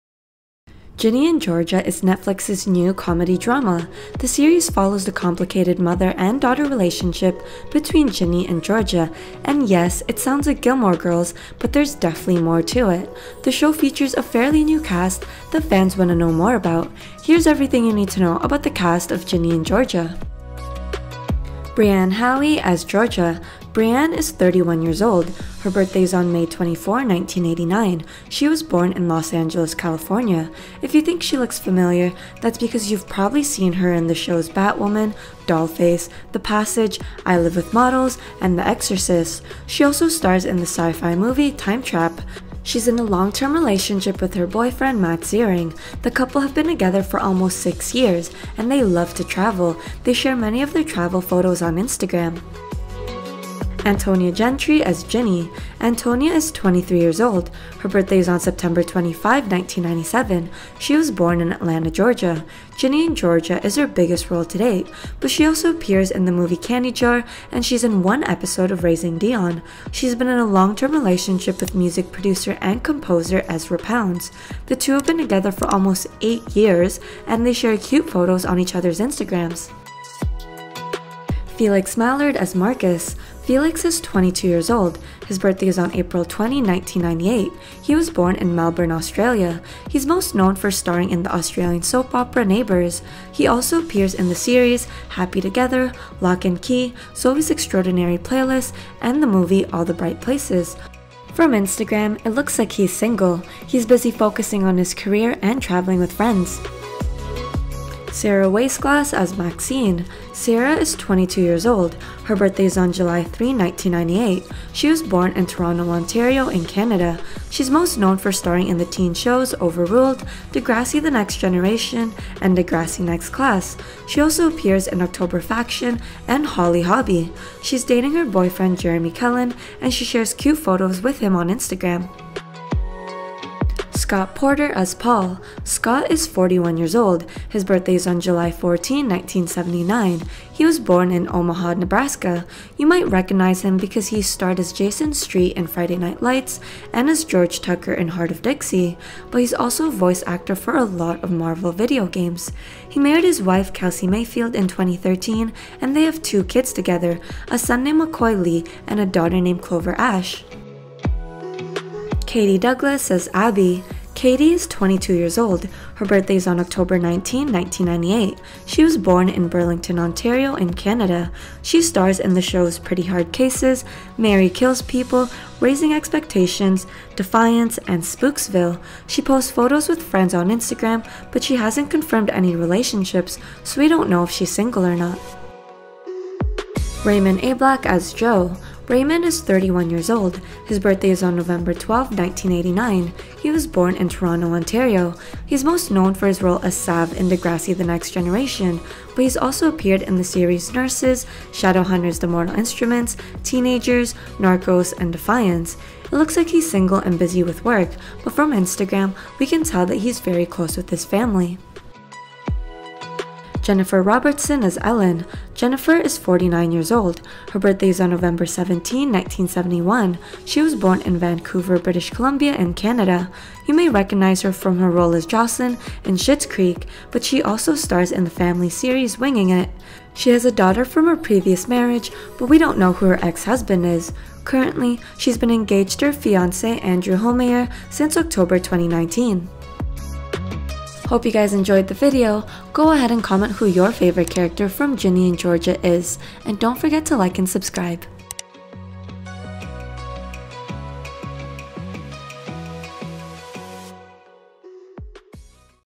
GINNY AND GEORGIA Cast: Real Age And Life Partners Revealed!
Ginny And Georgia is Netflix’s new comedy drama. The series follows the mother and daughter relationship between Ginny & Georgia, and it really caught fans by surprise. Fans also can’t get enough of the Ginny And Georgia cast. In this video we’re talking about the Ginny And Georgia cast real age and life partners revealed. How old are they? Who’s in a relationship? And much more. GinnyAndGeorgia.